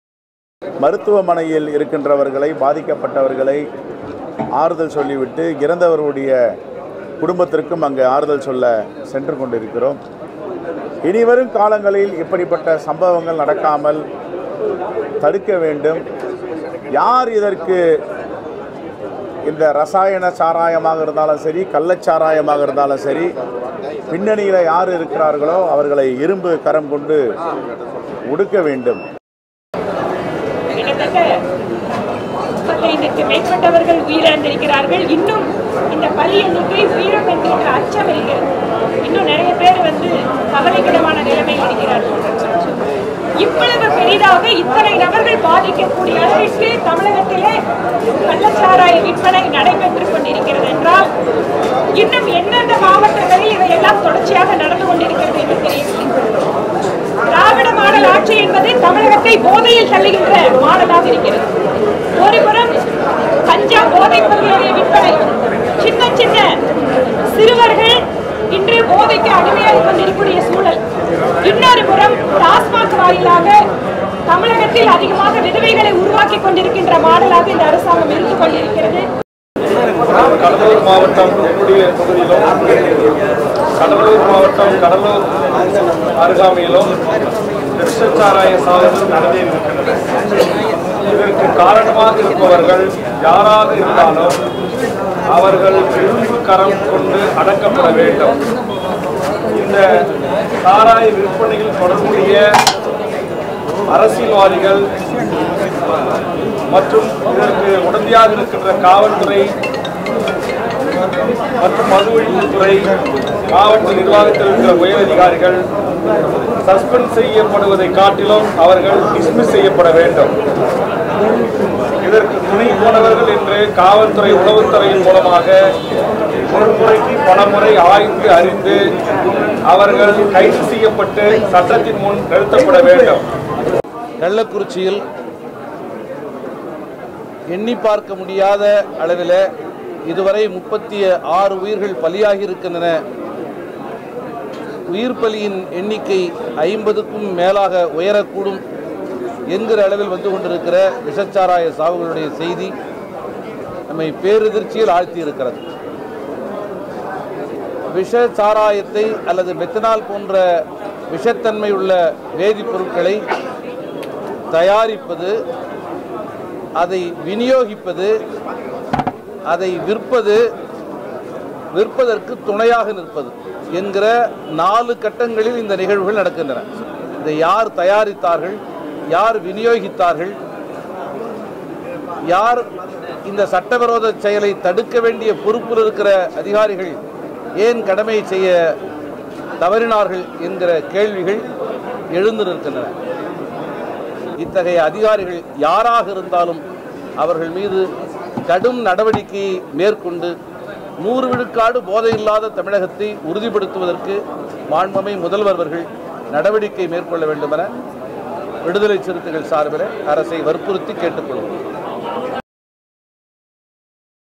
கள்ளக்குறிச்சி விவகாரம் - தலைவர்கள் கருத்து
கள்ளச்சாராய விவகாரம் தமிழகத்தில் பெரும்பிரச்சினையாக உருவெடுத்துள்ளது

இந்த விவகாரம் குறித்து பல்வேறு அரசியல் கட்சிகளின் பிரதிநிதிகளும் தங்கள் கருத்துக்களையும், வேண்டுகோளையும் முன்வைத்துள்ளனர்